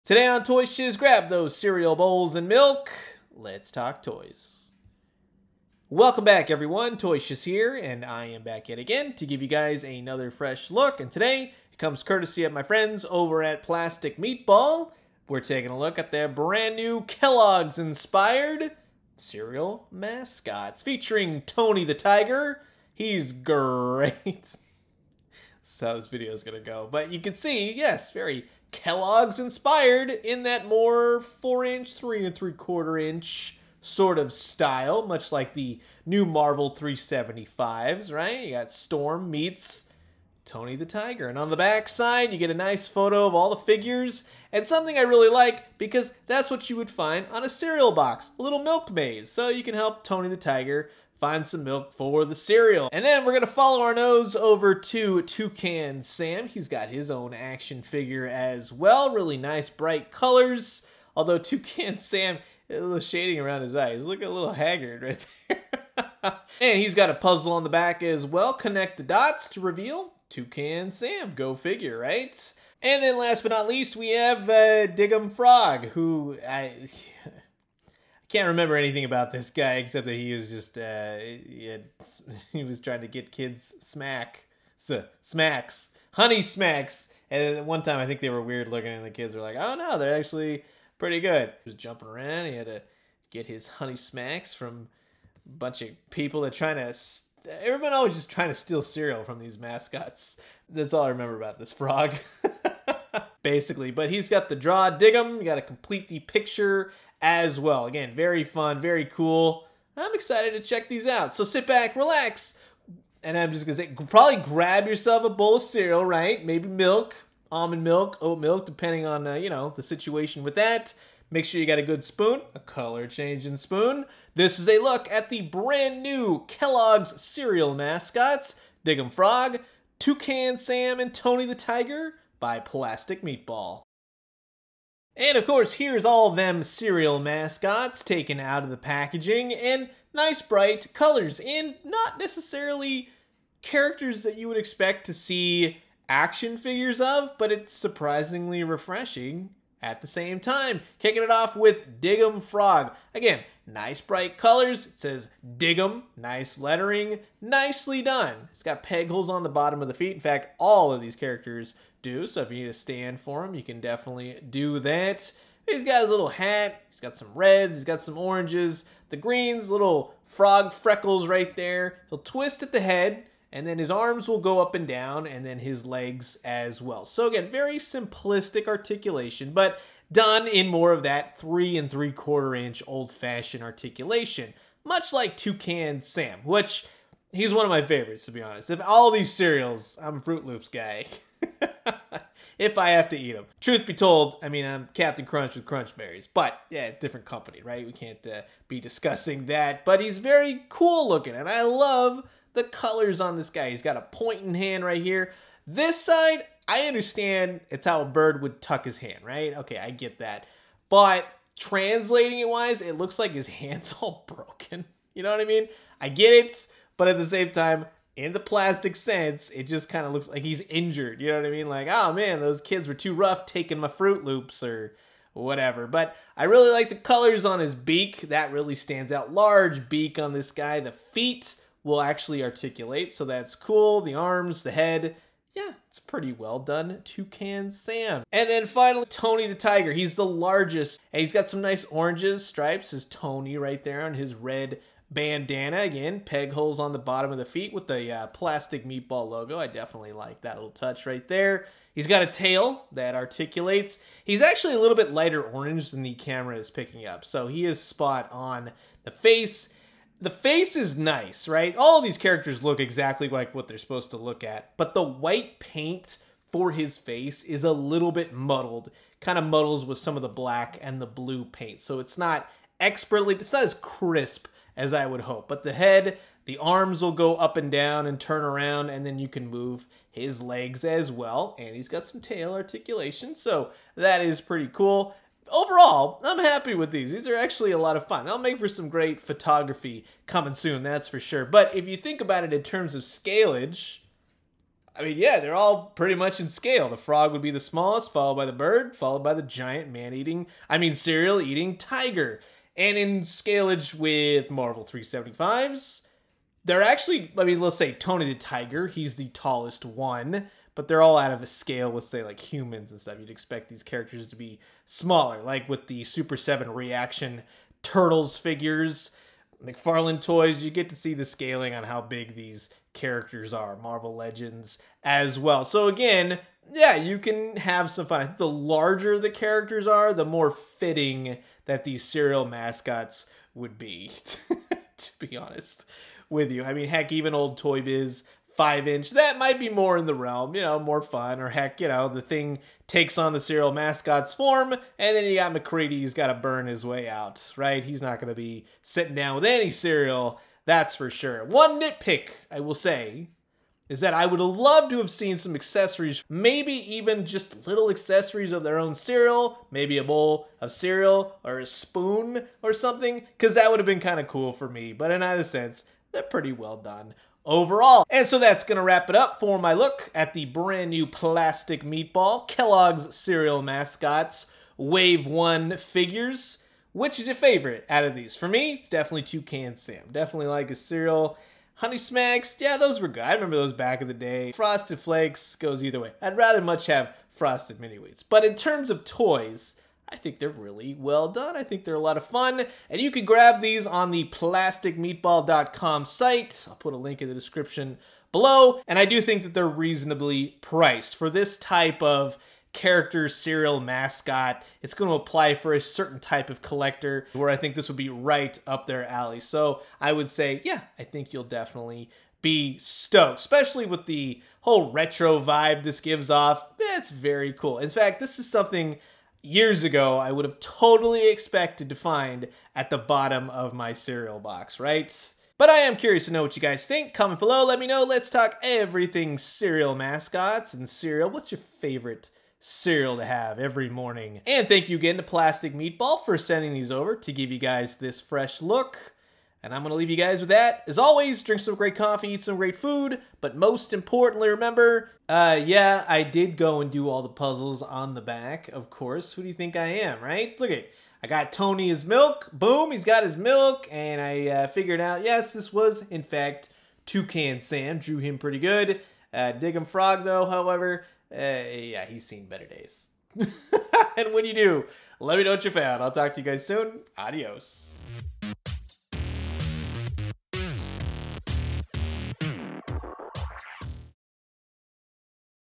🎃 2021 Kelloggs Retro Cereal Mascots | Wave 1 | Plastic Meatball Toys
Everything you need to know about the brand new Plastic Meatball created KELLOGGS Cereal Mascots action figures!

Want a set for yourself?
Checkout and check em out!